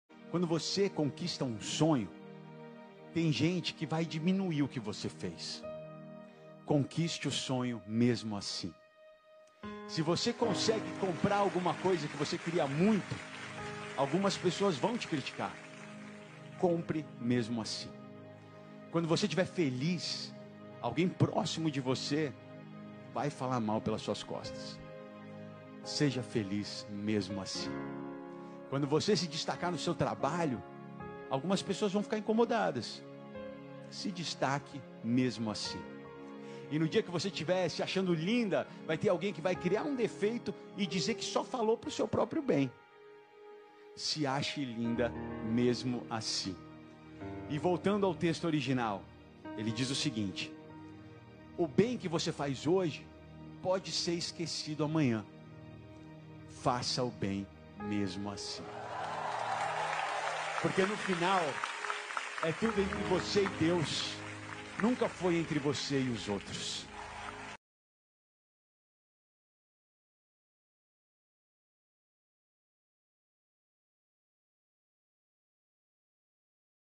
Dirigindo uma combi velha eu disse "Quero ter uma casa com vista para Serra Chapada" Deus ouviu 🙏🙏🙏
Dirigindo uma combi velha eu disse "Quero ter uma casa com vista para Serra Chapada" Deus ouviu... 15 anos após é só abrir a janela e olhar para o horizonte 🙏🙏